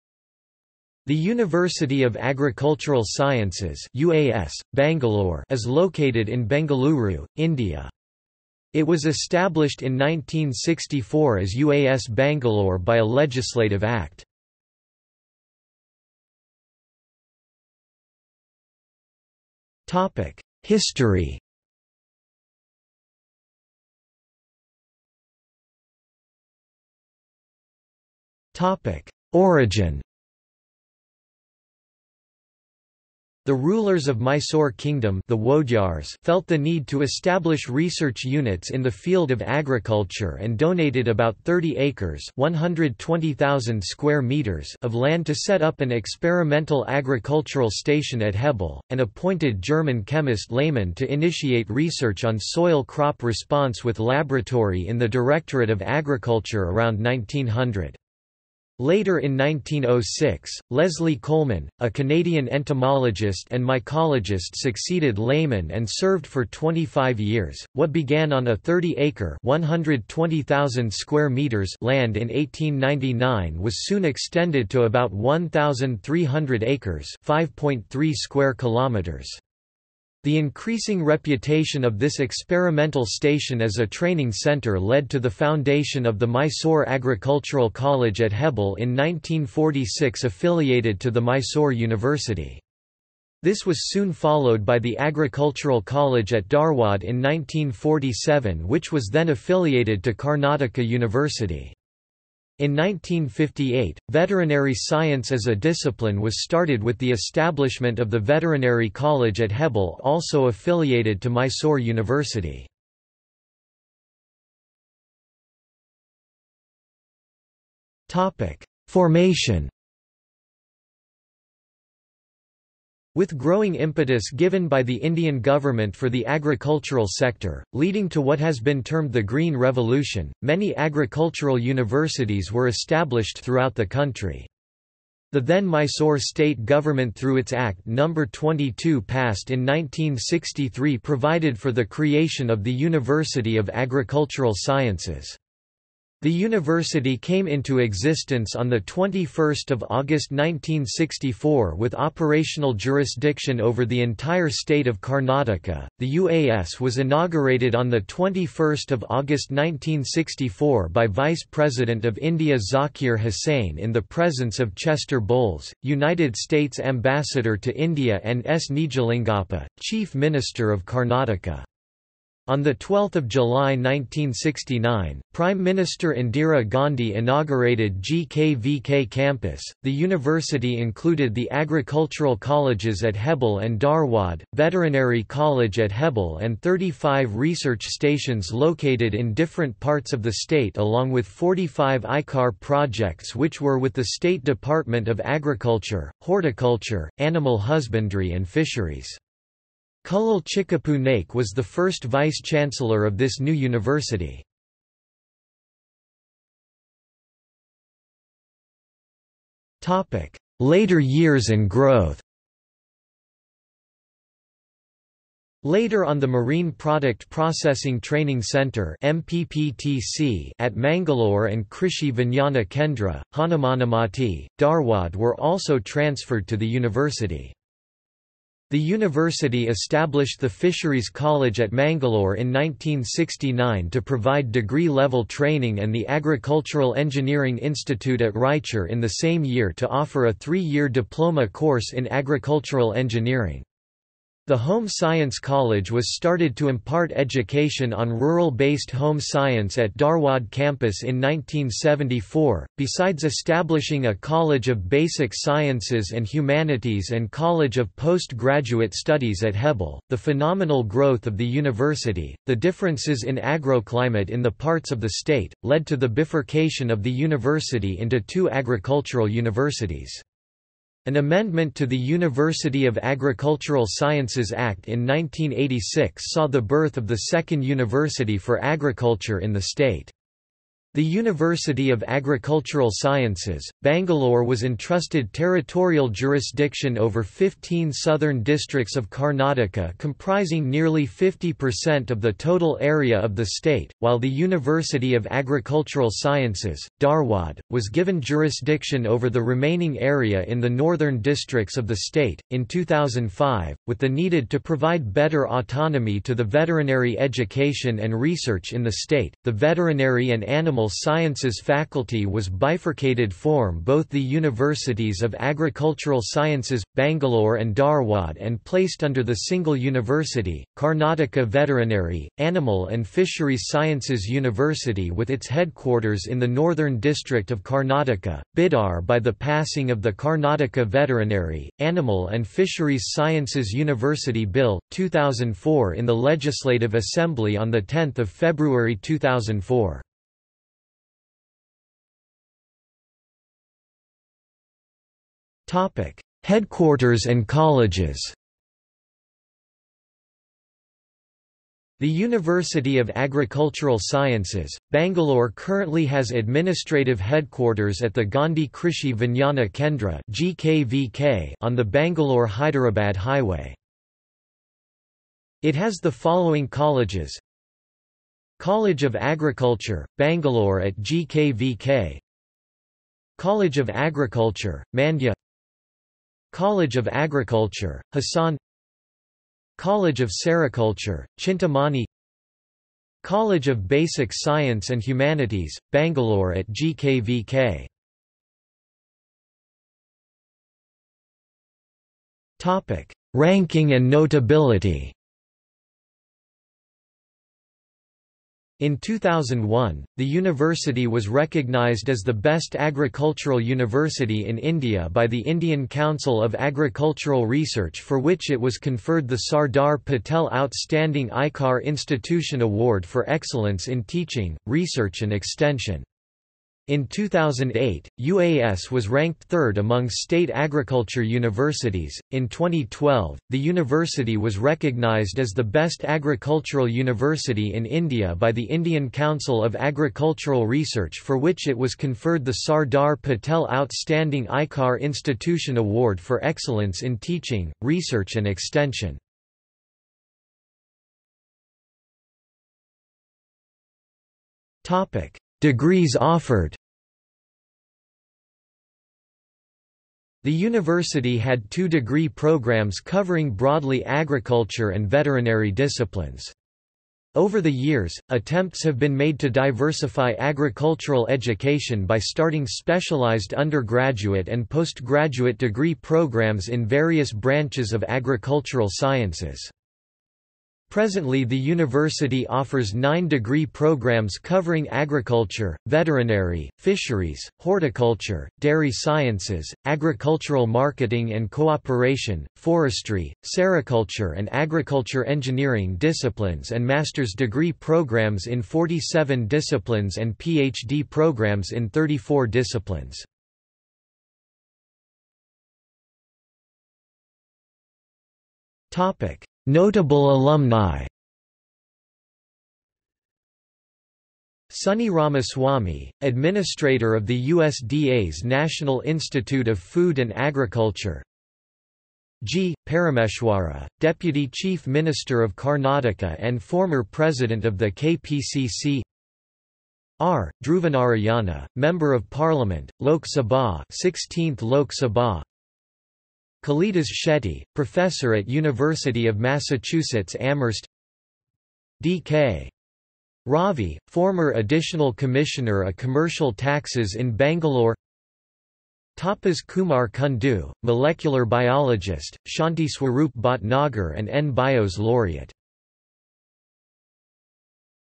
University of Agricultural Sciences, Bangalore | Wikipedia audio article
00:00:16 1 History
00:00:25 1.1 Origin
00:01:54 1.2 Formation
00:03:26 1.3 Later years and growth
00:05:42 2 Headquarters and Colleges
00:06:29 3 Ranking and notability
00:07:21 4 Degrees offered
00:08:15 5 Notable alumni

- Socrates

SUMMARY
The University of Agricultural Sciences (UAS, Bangalore) is located in Bengaluru, India. It was established in 1964 as UAS Bangalore by a legislative act.